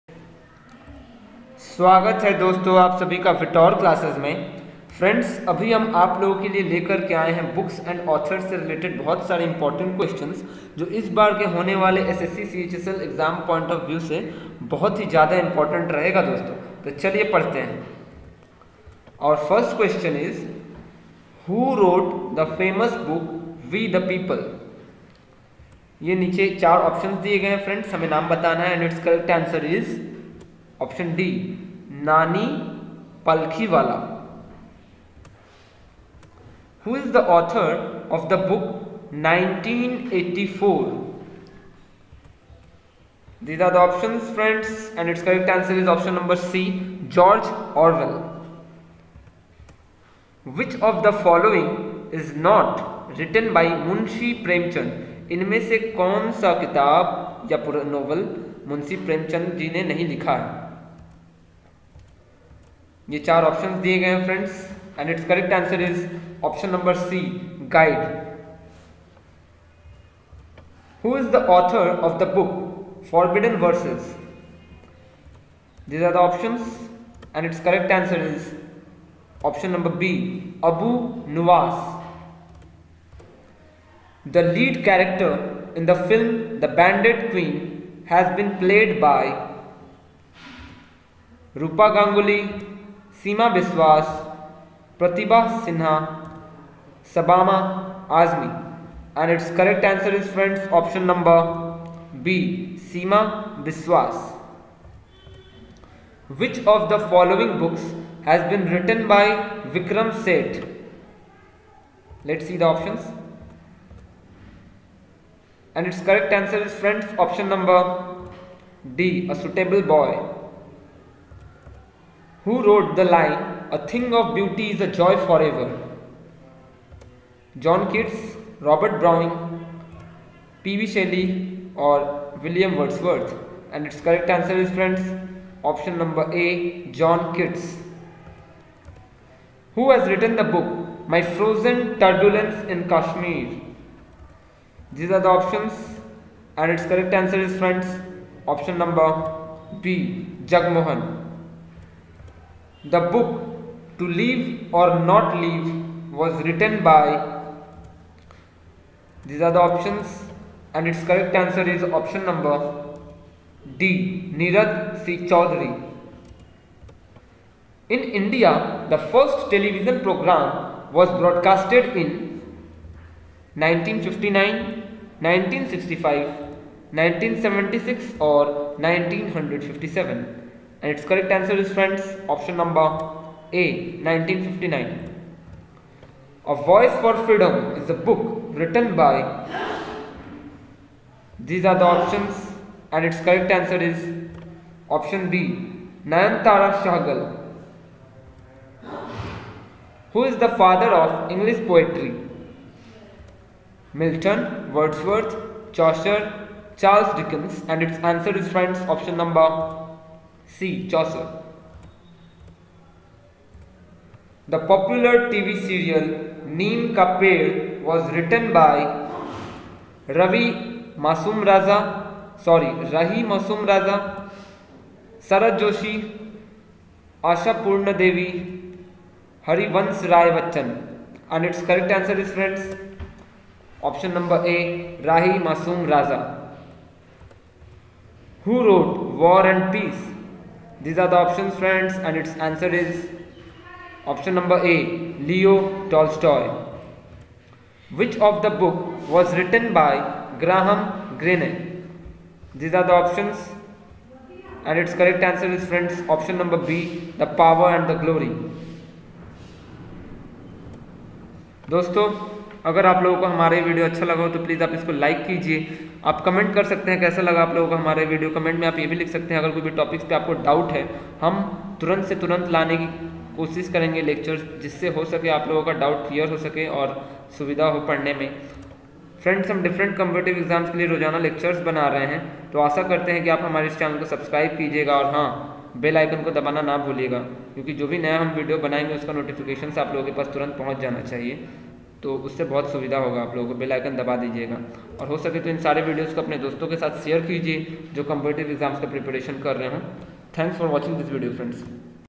SSC CHSL 2018 GK || BOOKS AND AUTHORS || MUST WATCH
Learning about chsl is very important and you must learn gk for sscchsl 2018. Books and authors are very important part of it.